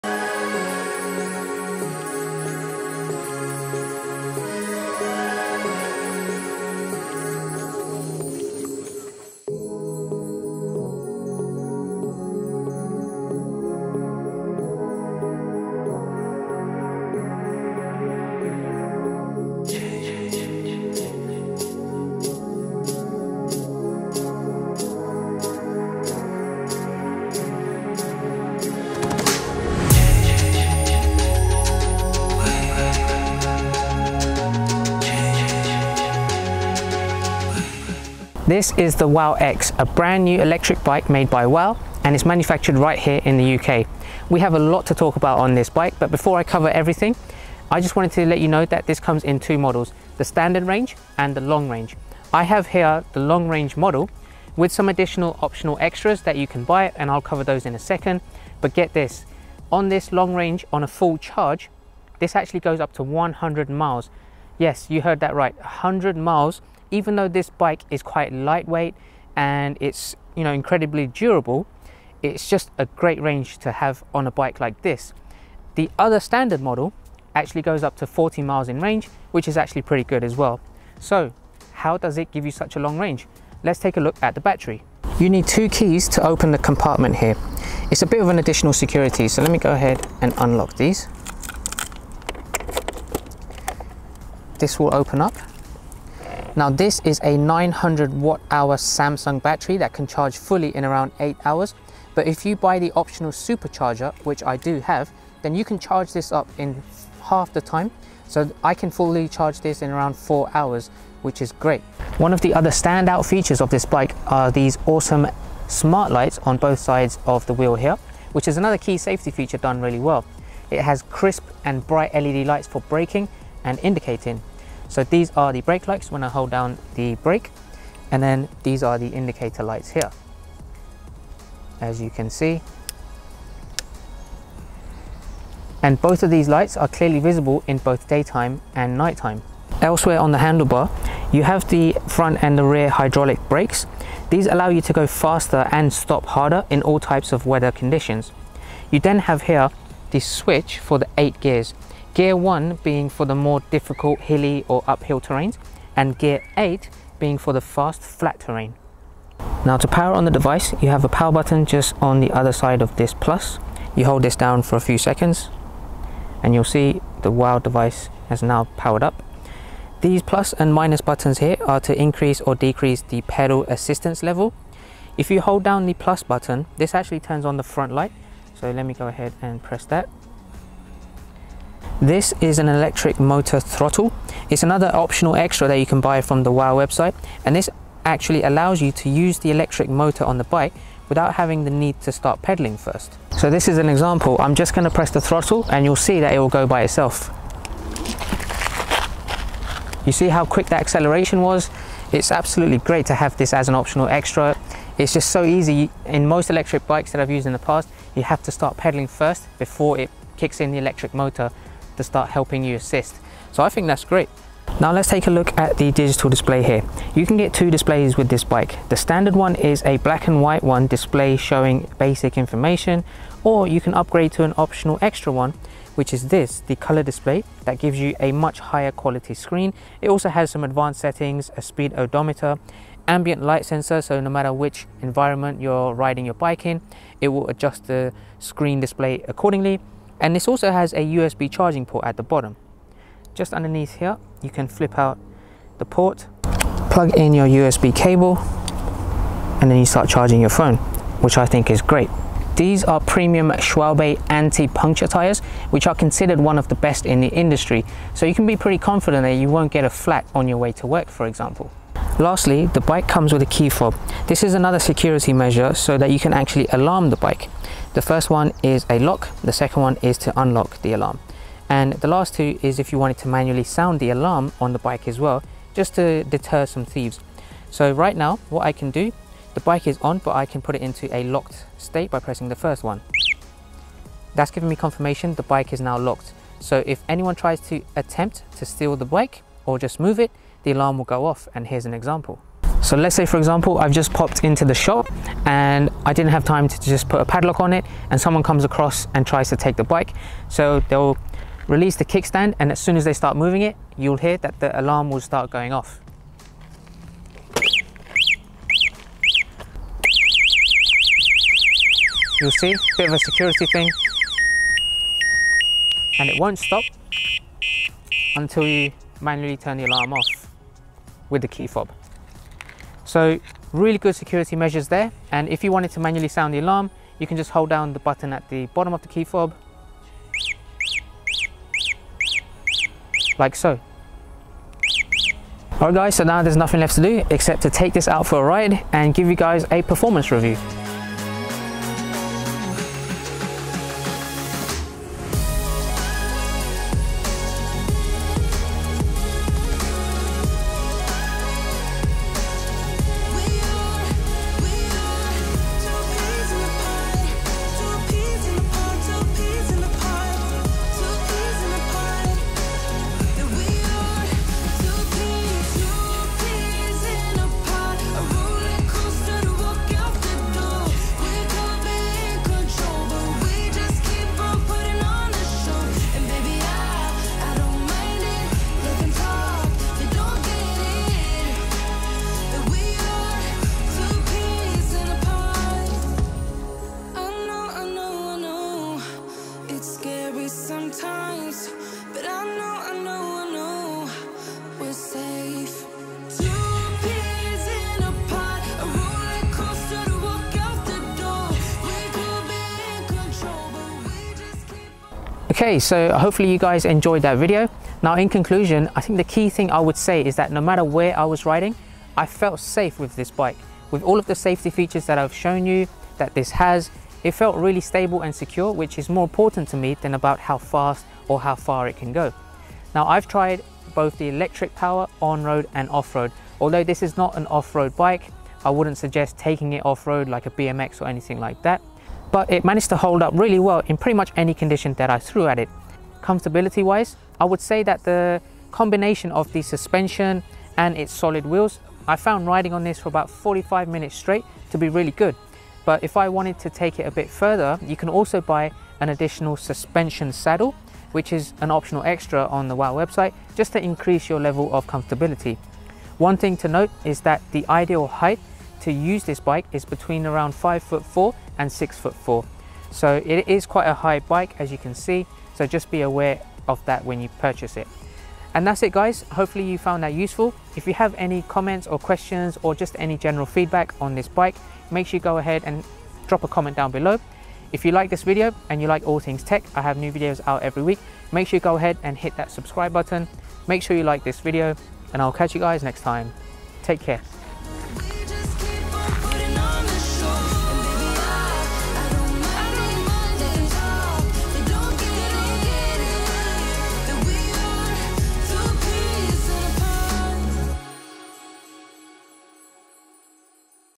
Introducing - The WAU X PLUS - A 100 Mile Range Electric Bike with Smart Lights! (Inc Discount)!
This is the brand new electric bike from WAU - it's the WAU X! It has 2 models, with the top model going up to 100 miles in range, has incredible lighting with indicators and plenty of optional extra's to give you that boost!

Here are the main improvements for WAU X Plus:

• Increased battery range - 900Wh (100 miles)  (Previous Gen: WAU X - 882Wh)
• Improved battery management chip with added insulated housing – Which gives better energy consumption and longer life (Previous Gen: WAU X: Older manajemen chip)
• Upgraded Motor – Bafang 250-350W peak  (Previous Gen:  WAU X - TruckRun 250W)
• Industry Best - Schwalbe tyres 48mm  (Previous Gen: WAU X - Vee Tire)
• Front Light – Fully integrated front light, draws power directly from the battery pack.  (Previous Gen:  WAU X - Detachable USB Light, with stand alone battery)
• Reinforced Wheels – Reinforced double-walled aluminium rims with double crossed pattern  (Previous Gen:  WAU X - Single crossed pattern)
• New V3 GPS Tracker – Latest generation of the tracking device. (Previous Gen:  WAU X - V2 )
• WAU custom mudguards - Come as standard with all refreshed models.   (Previous Gen:  WAU X - Mudguards not included as standard)
• As well as many other components and mechanical parts upgraded.

FREE MUSIC FOR CONTENT

MY FILMING ACCESSORIES:
4K Monitor
Studio Ring Light